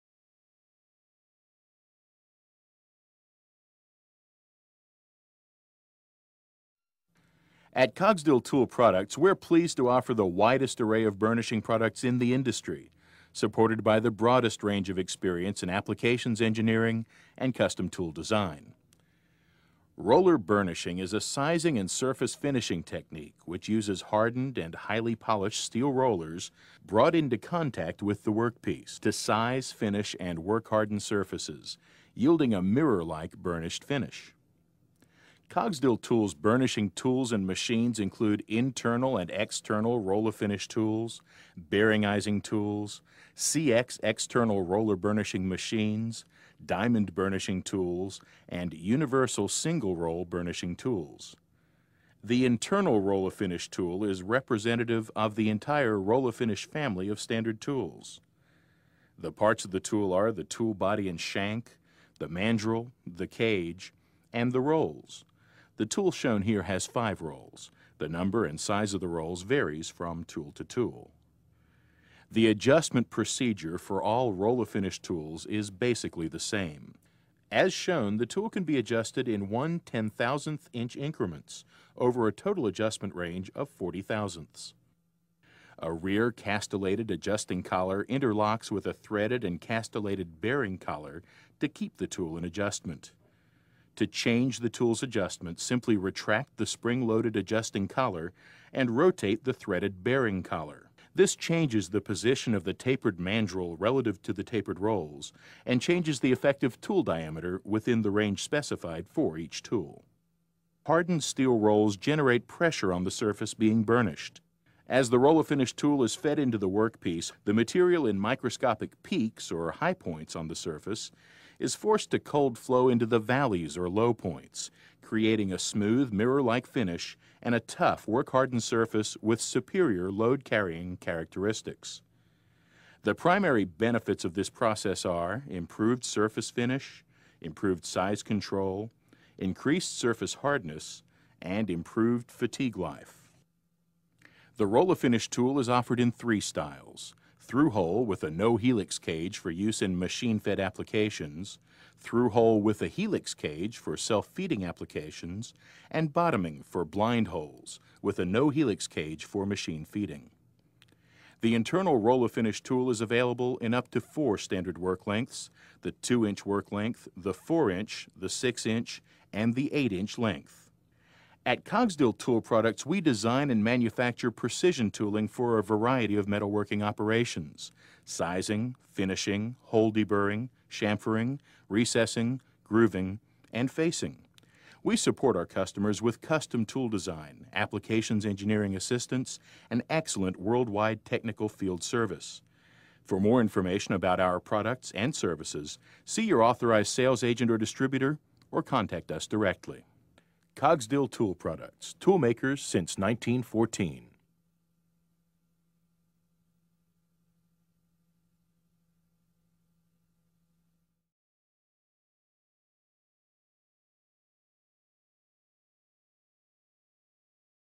Cogsdill Roller Burnishing
Cogsdill Nuneaton and Cogsdill Tool Products offer the largest range of Roller burnishing Tools in the Industry.  Internal, External, diamond, multi-roll, single roll and external roller burnishing machines.